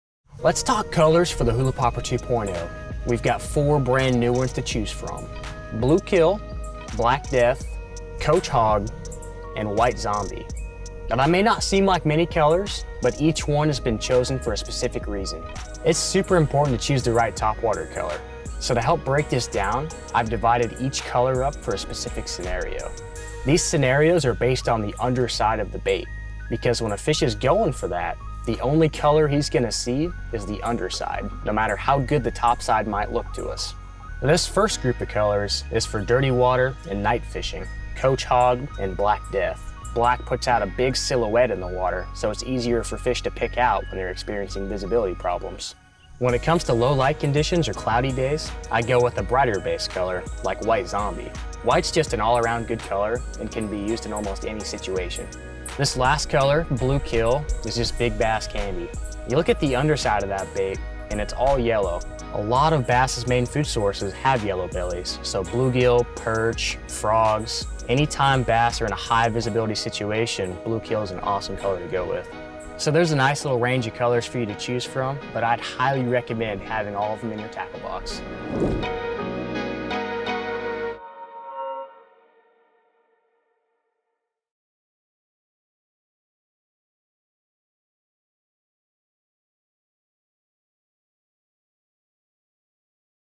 Hula Popper 2.0 | Choosing A Color
The Arbogast Hula Popper 2.0 features four new colors: Black Death, Coach Hog, Blue Kill, and White Zombie. The lineup covers all the bases: darker based colors for creating distinct silhouettes, a bright color for overcast conditions, and a bluegill imitator for topwater pond predators. Ethan Butler breaks down this color lineup, and explains why each one works in its own specific fishing scenario.